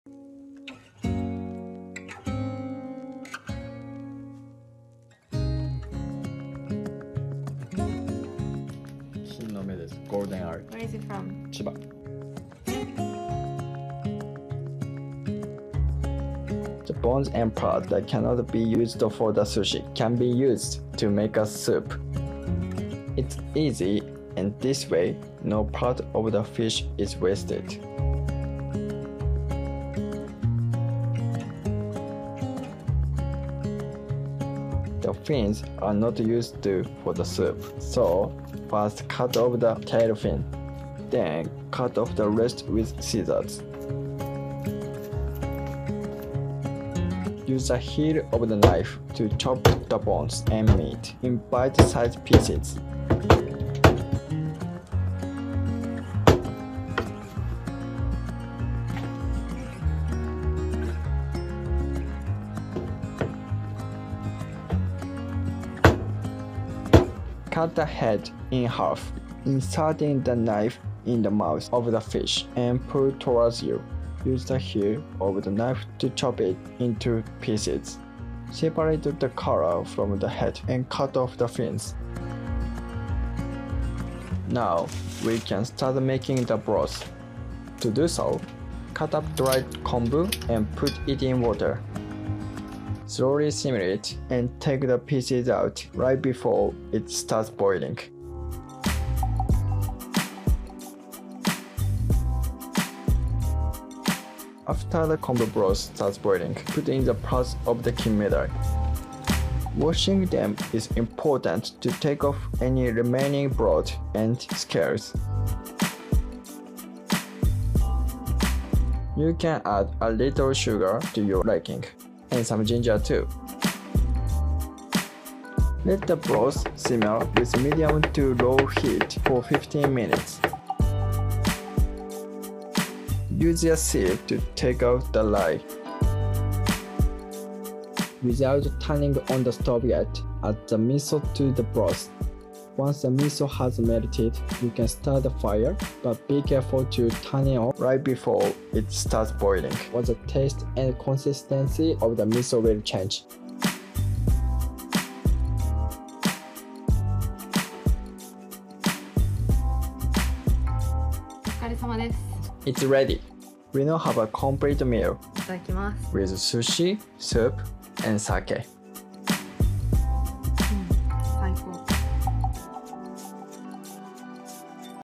Japanese Sushi Chef vs Miso Soup (Kinmedai)
Miso soup is one of the staples of Japanese cuisine and can be very delicious if you know how to make it.
When making sushi, you can use the parts of the fish that are not used for the sushi to make the broth fro the miso soup.
It’s easy and this way no part of the fish is wasted!